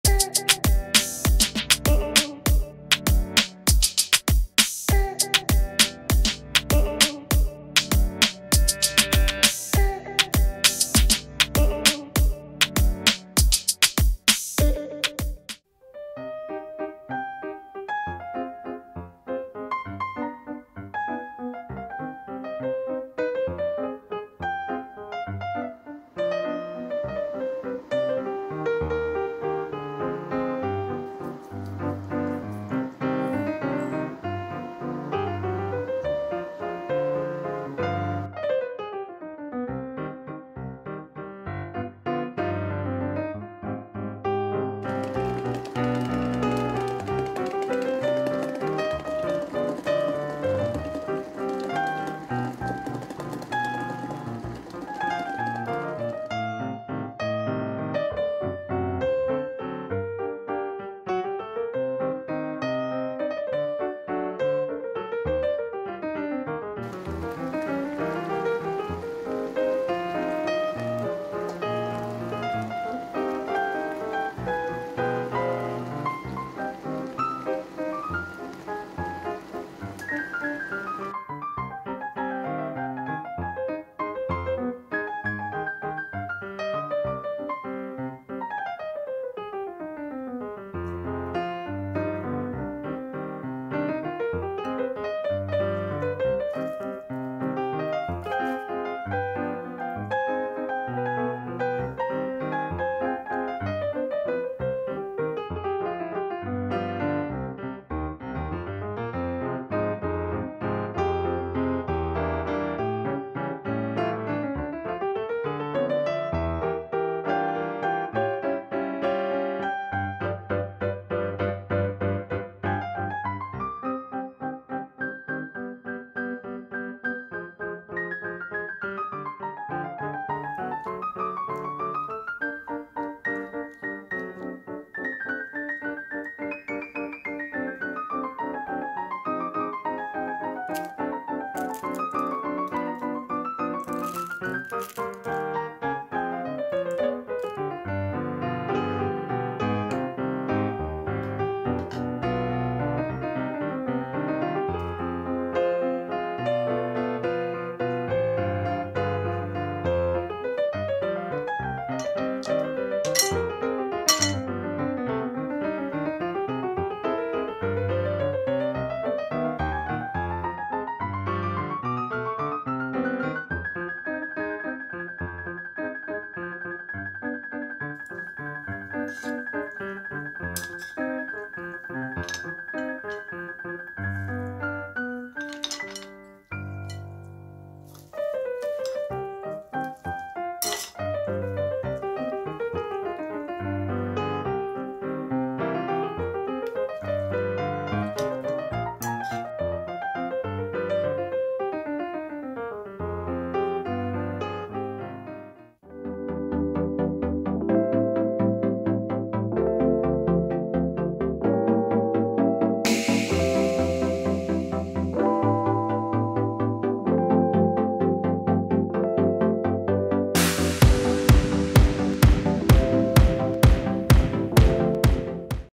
Spicy Bibim Noodles Recipe / Korean Food / 비빔면 맛있게 요리하기
Bibim noodles or spicy noodles, a cold dish made with very thin wheat flour noodles called somyeon with added flavorings, is one of the most popular traditional noodle dishes in Korean cuisine and especially popular during summer.

Menu: Bibim Noodles